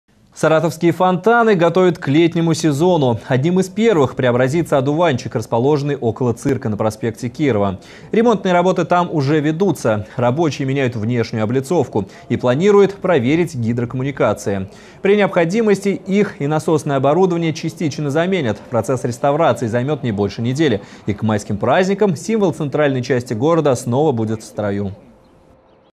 Саратовский фонтан "Одуванчик" готовят к летнему сезону
Саратовские фонтаны готовят к летнему сезону. Одним из первых преобразится "Одуванчик", расположенный около Цирка на проспекте Кирова. Ремонтные работы там уже ведутся. Материал вышел в эфир 24 апреля 2015 года.